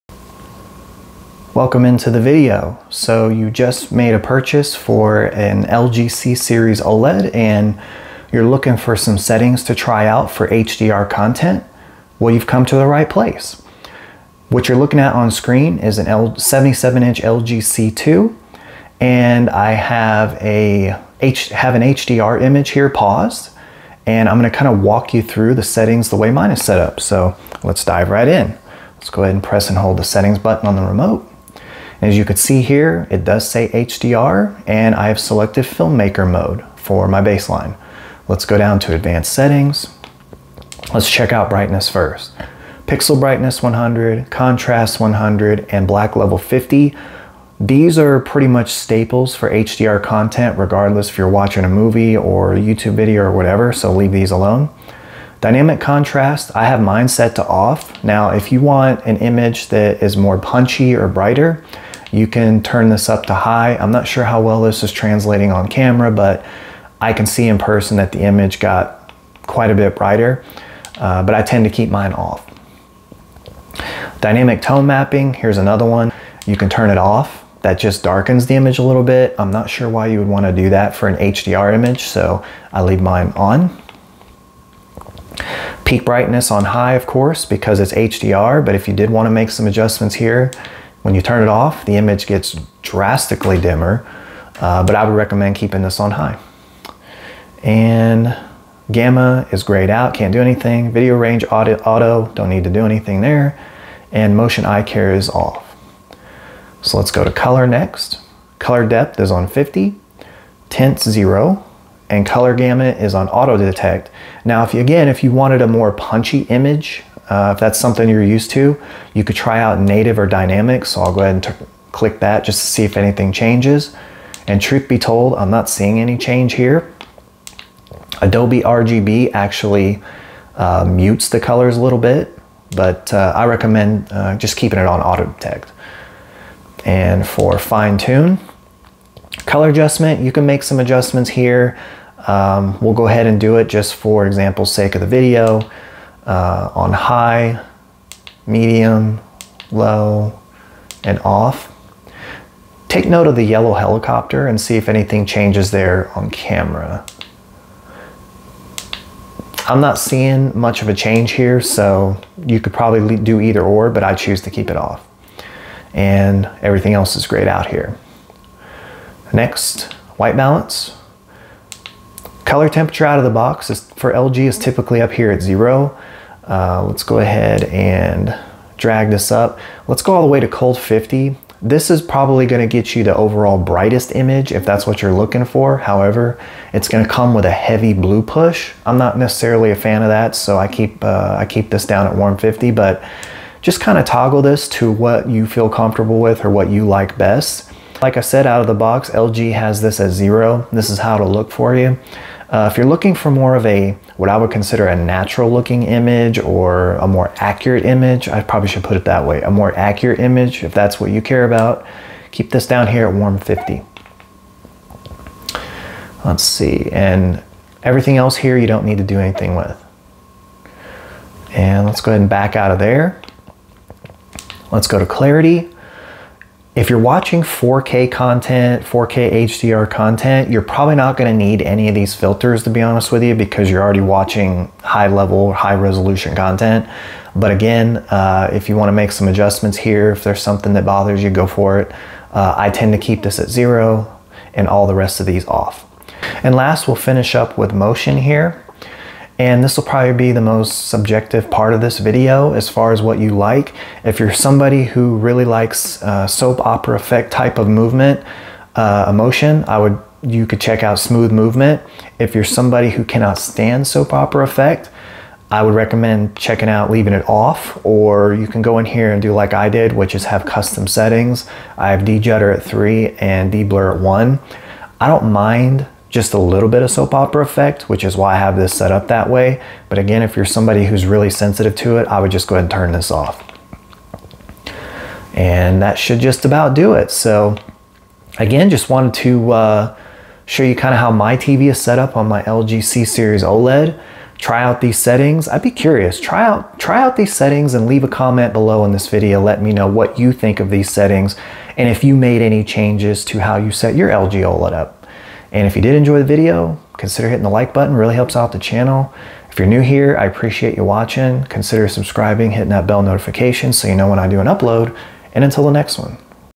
HDR Movie Settings For Your LG OLED! | LG C2 | Apple TV 4k (2022)
In this video, I go over some settings to tryout for HDR content on the LG C1/C2/C3 series OLED!